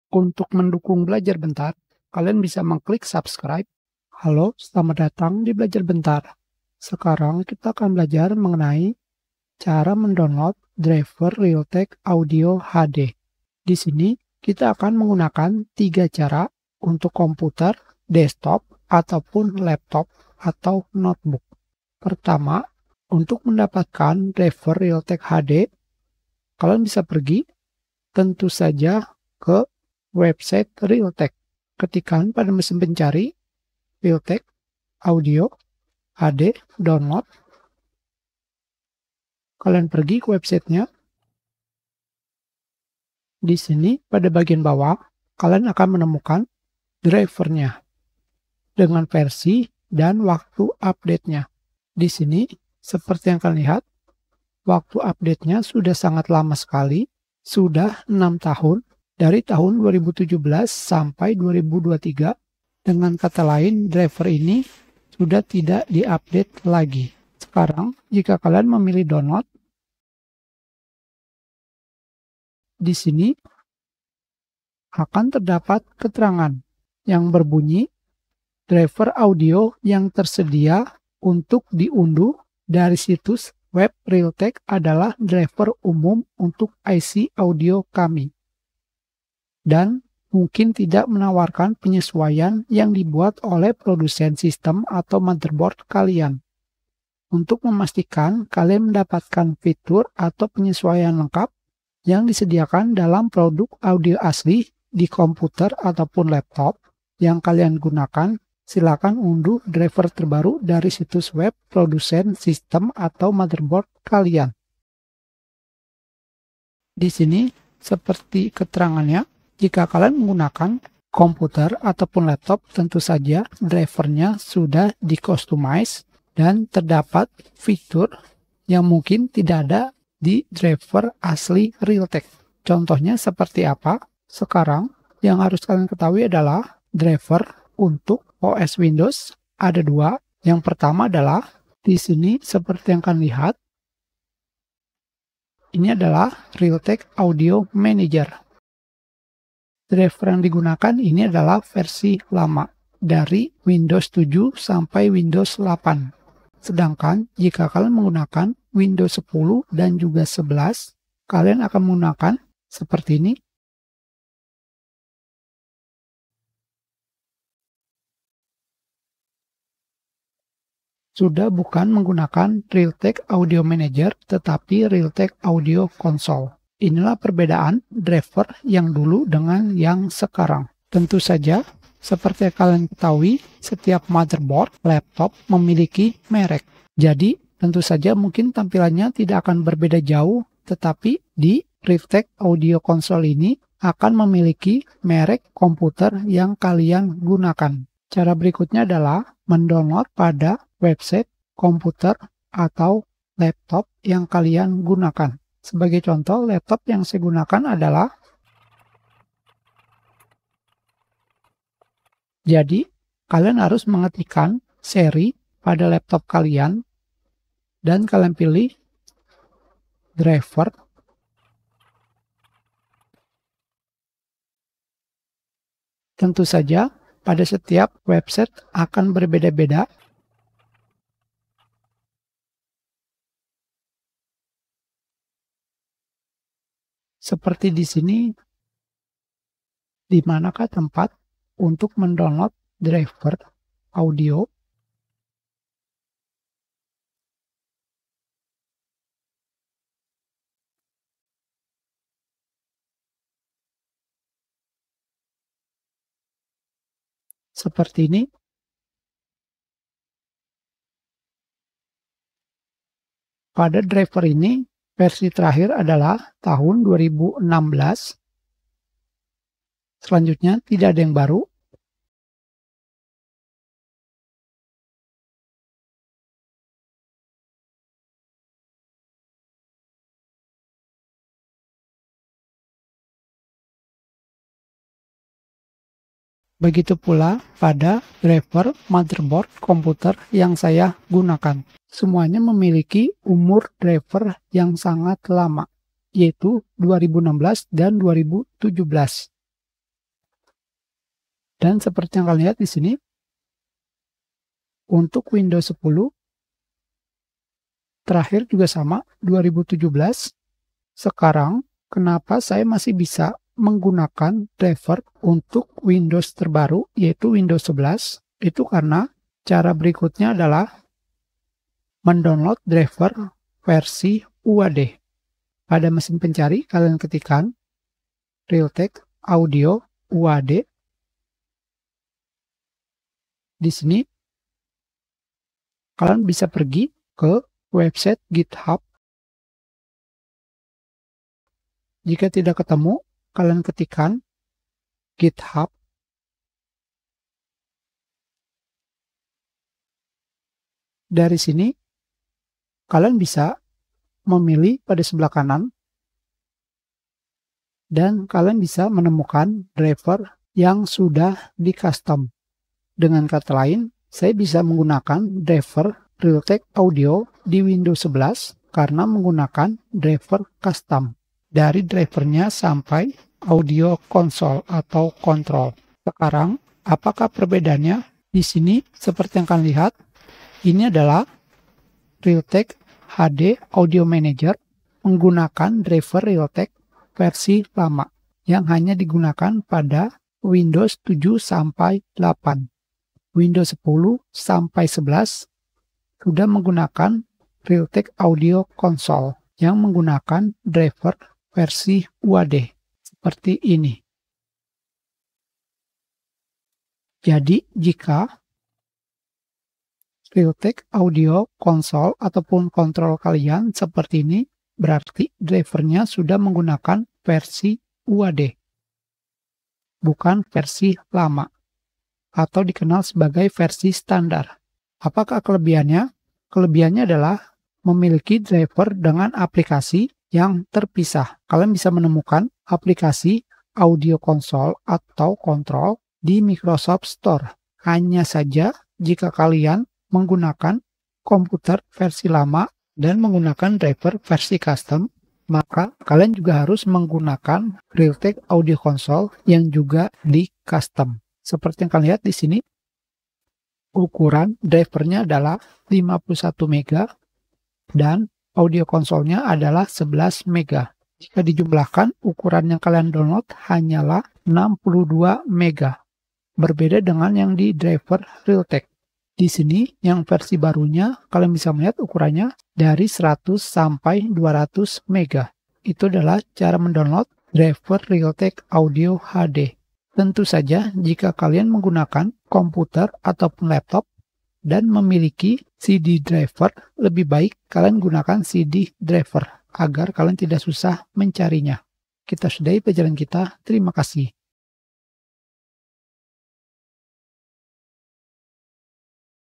3 Cara Download Driver Realtek Audio HD Untuk Windows 10 dan Windows 11
Sekarang kita akan belajar mengenai cara mendownload drivers Realtek Audio HD. Apakah kegunaan driver realtek Audio HD? digunakan untuk memaksimalkan suara yang dihasilkan dari komputer ke speaker. Pada Windows 10, Windows 11 sudah memiliki driver audio generik yaitu driver audio Microsoft, yang sayangnya tidak bisa menghasilkan suara yang kencang jika dibandingkan dengan driver audio Realtek.

Sekarang driver realtek memiliki 2 jenis yaitu Standar dan UAD/DCH. Versi standar digunakan pada Windows 7, Windows 8. Sedangkan versi UAD untuk versi Windows 10 dan Windows 11.

Untuk bisa mendapatkan driver audio Realtek kalian bisa mendownload dari 3 tempat yaitu:

00:00 Cara Download driver Realtek Audio HD dari Website Realtek
03:16 Cara Download driver Realtek Audio HD dari Website Merek Komputer,  Laptop/Notebook
04:20 Cara Download driver Realtek Audio HD UAD Custom